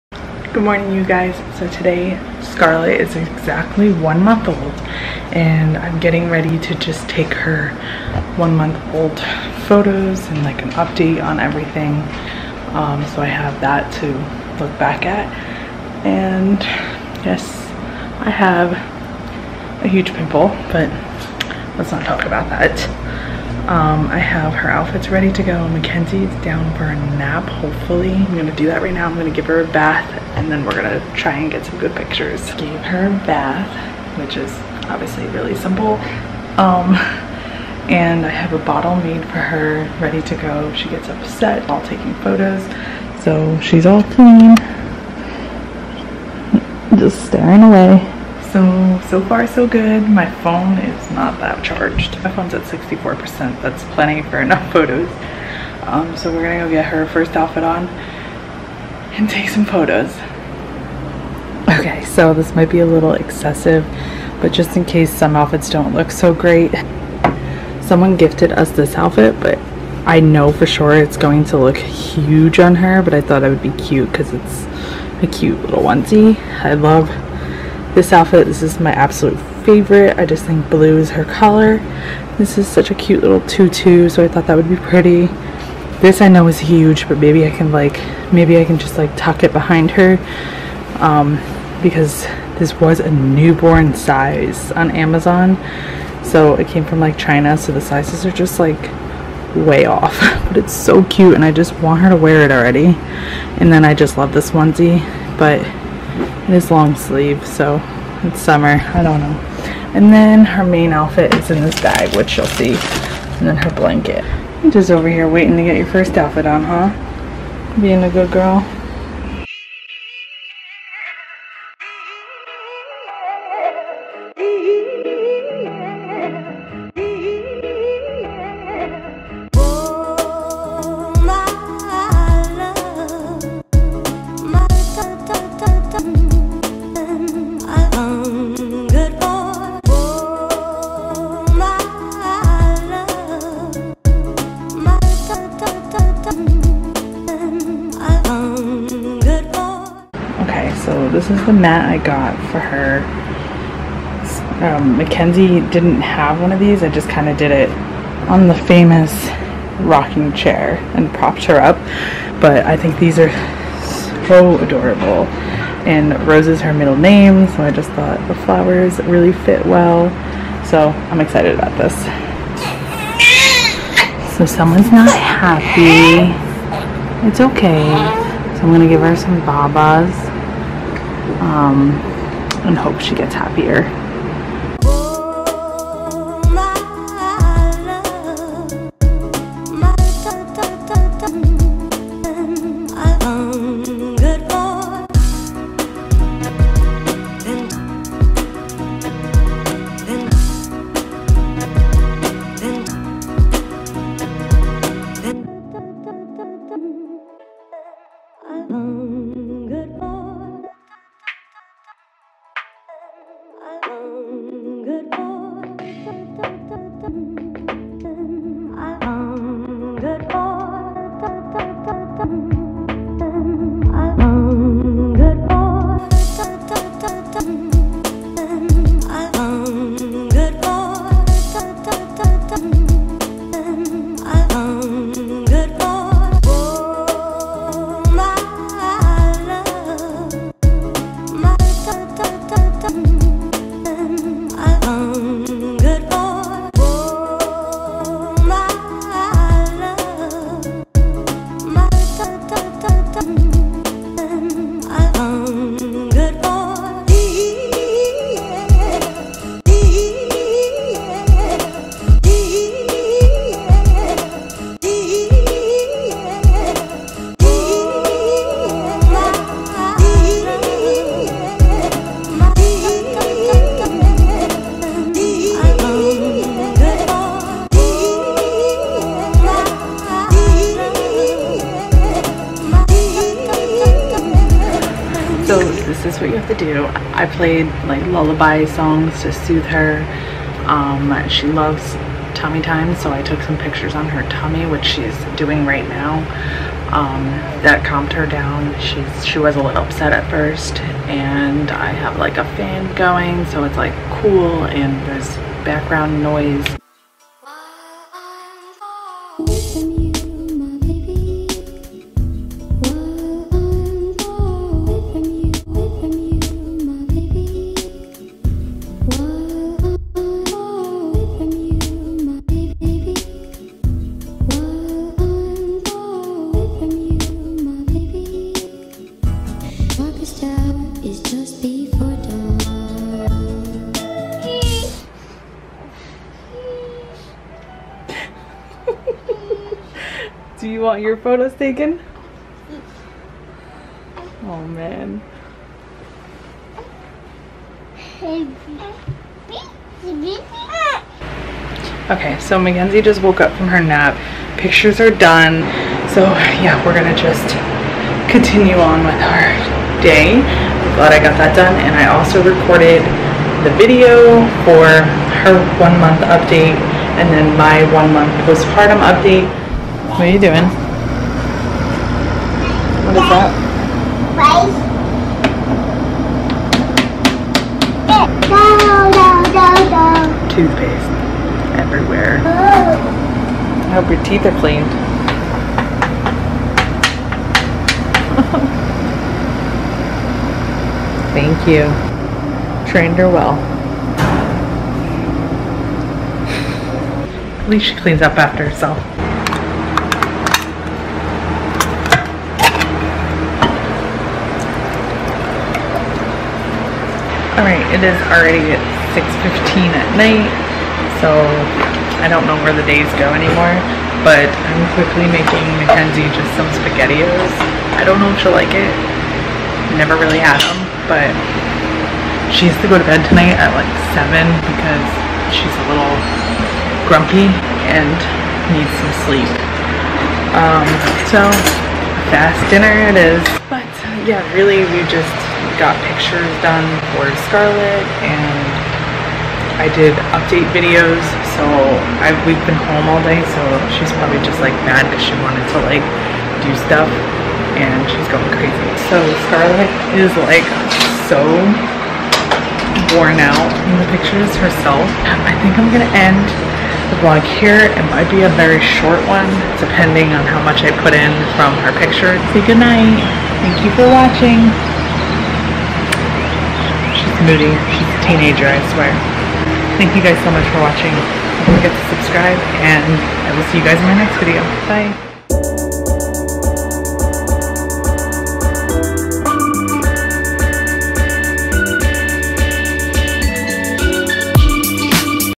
DIY One Month Old PhotoShoot| Baby Milestones
i am taking scarletts milestone photos at home with my cellphone and i bring you along behind the scenes!

We upload 3x a week ! Come along and follow our journey !